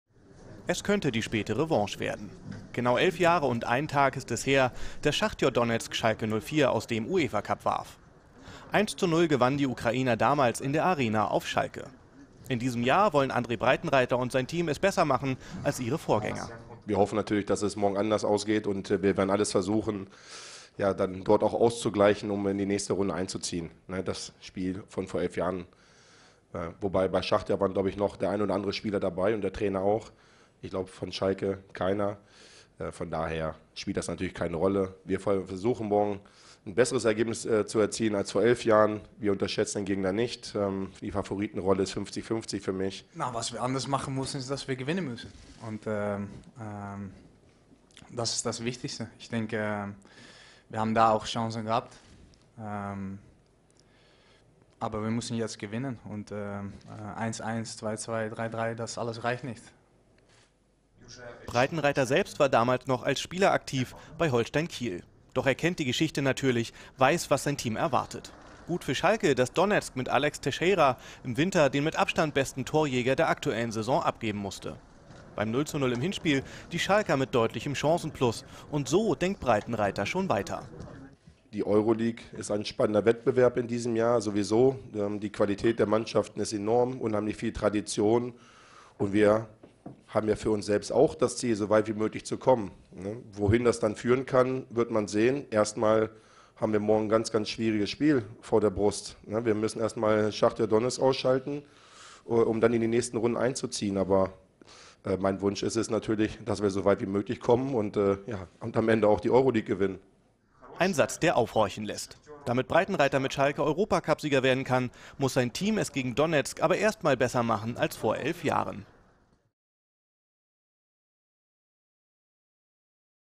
Breitenreiter: "Besseres Ergebnis als vor elf Jahren" - kicker.tv
Breitenreiter: "Besseres Ergebnis als vor elf Jahren"

Vor fast genau vor elf Jahren warf Schachtar Donezk den FC Schalke 04 aus dem Europapokal. Das wollen die Königsblauen am heutigen Donnerstagabend besser machen und nach dem 0:0 im Hinspiel eine Runde weiter ziehen. Und wenn das geschafft ist? "Dann wollen wir so weit wie möglich kommen und die Europa League gewinnen", so der Coach der Knappen.